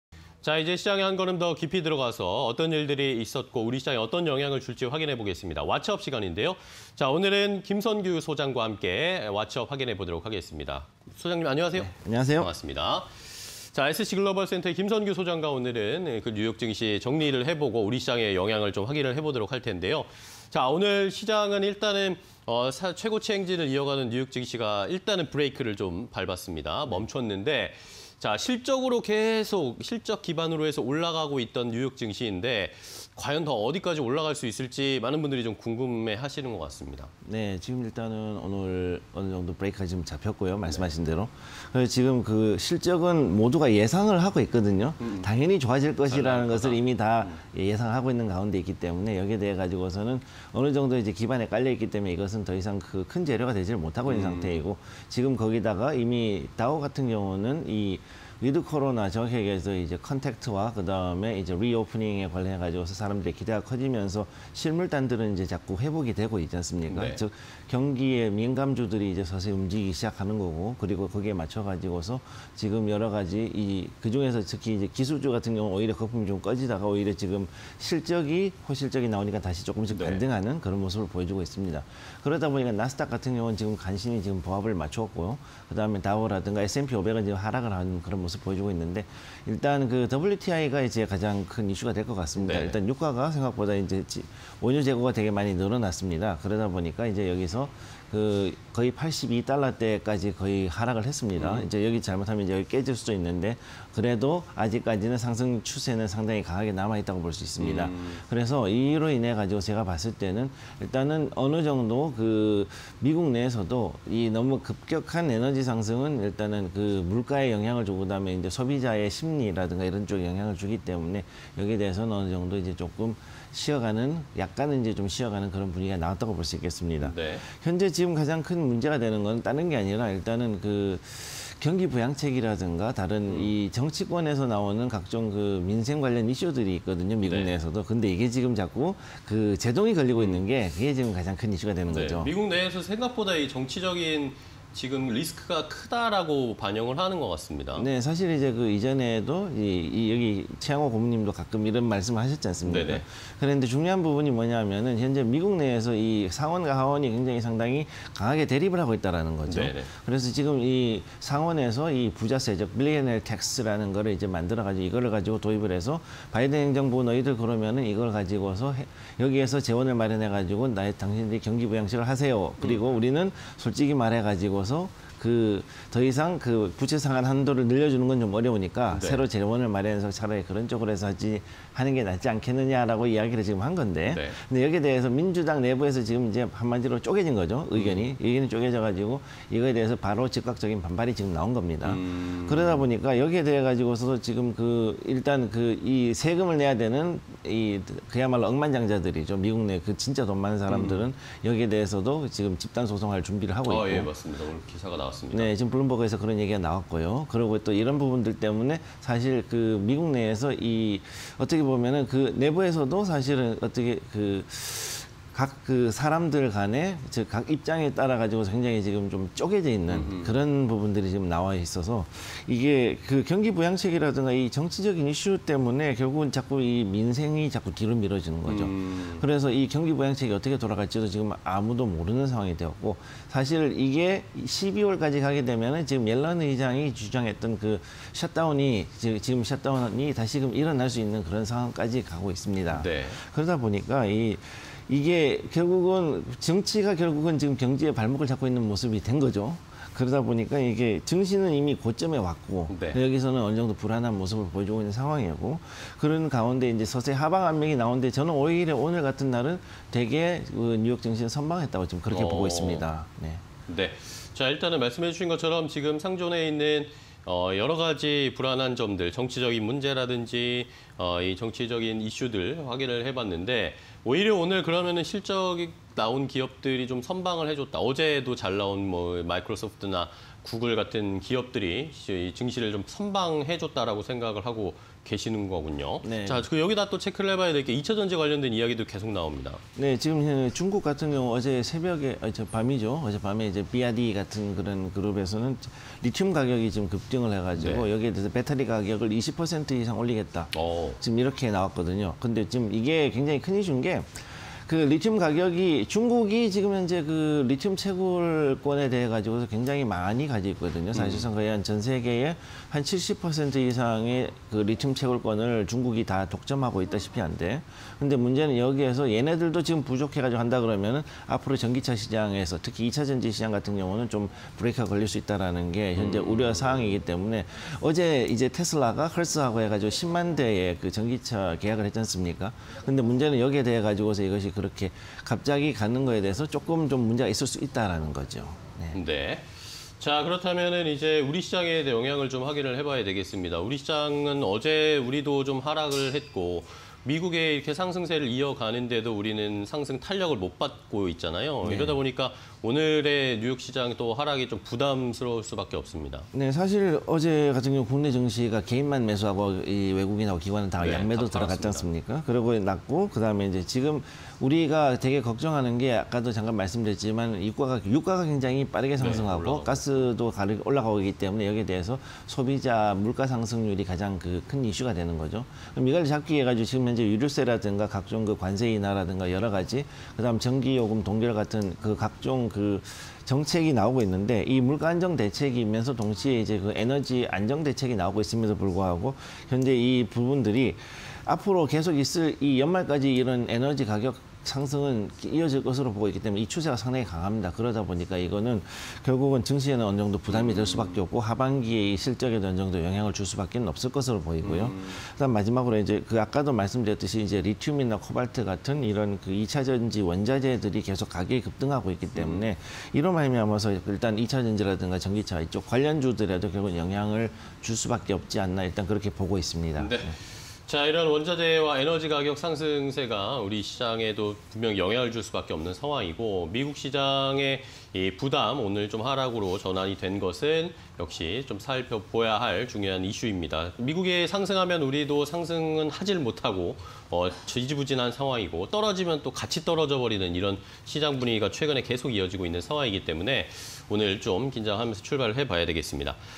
실적만으로는 버틸 수 없는 증시, 앞으로의 향방은?_김선규의 왓츠업 (20211028)
- 美 민주당, '부자세' 도입 즉각 반발
- 경기부양책 '오리무중'
- 美 의회, 각종 정치적 이슈로 민생 뒷전
- 美 증시, 이미 고점에서 불안한 모습… 하방 압력 확대
- 中 비야디, 리튬 가격 급등에 배터리 가격 20%↑
- 2차전지에 '빨간 불'
- 국내 증시, 전일 외국인·기관 양매도에 하락 마감
- 유류세 인하 등 물가 안정 대책에도 에너지 가격 상승
- 리튬·코발트 등 2차전지 원자재 가격 급등